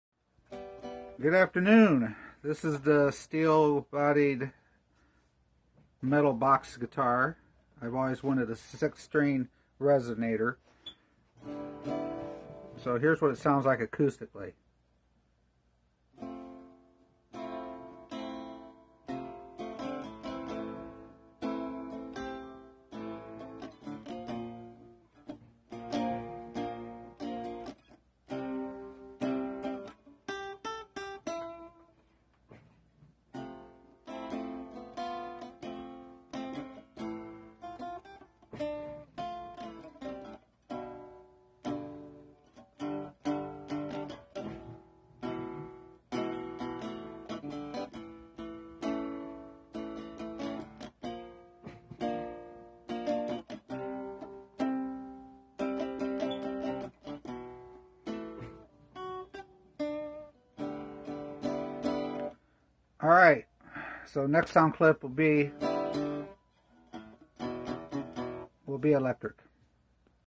HobbyLobbyReso1
6 string, acoustic/electric, 22.25" scale, black ice single coil pickup, volume, internal springs, first act neck, walnut bridge, reso tailpiece, shower drain sound hole cover